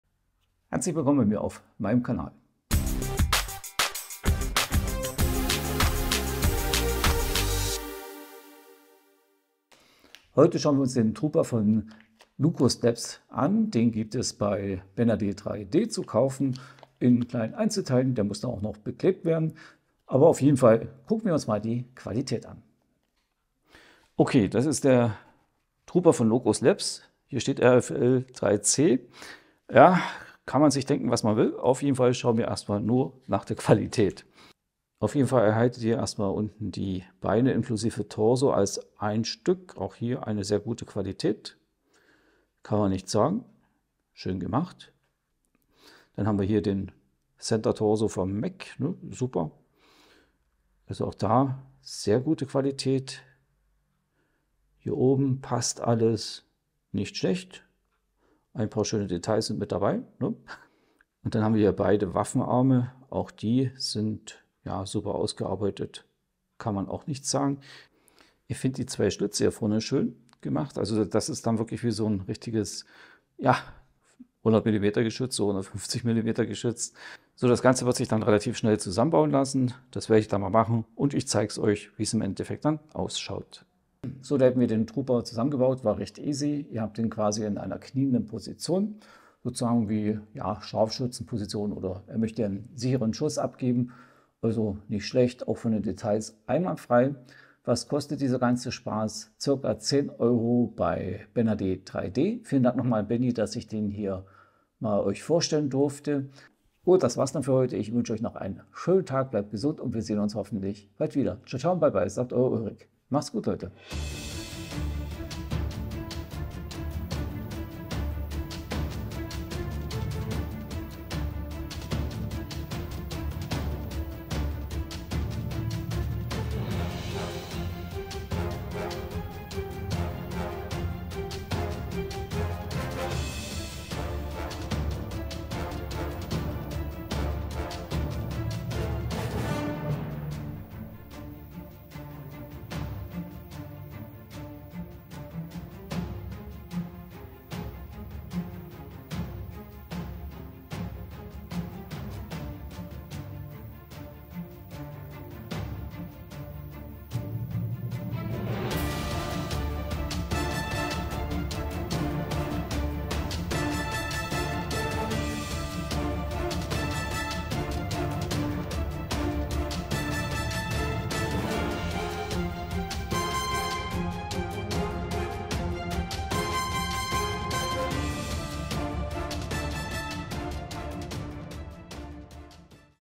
Der Trooper / Riflemen von Locust Labs
Der Trooper von Locust Labs ist eine 6mm Miniatur, die für Battletech, Hardwar und ähnliche Spielsysteme in diesem Bereich geeignet ist. Zusätzlich gibt es eine passende Base, so dass nicht nur die äußerliche Verwandschaft zum Rifleman erkennbar ist, nein man kann den sofort für Battletech proxen.

Anmerkung: Den Trooper gibt es kniend, stehend, kampfbereit und in weiteren Posen.

Vielen Dank an BenAD3d für diesen Mech zum "Begutachten". Also, schauen wir uns Optik und Quali fix an.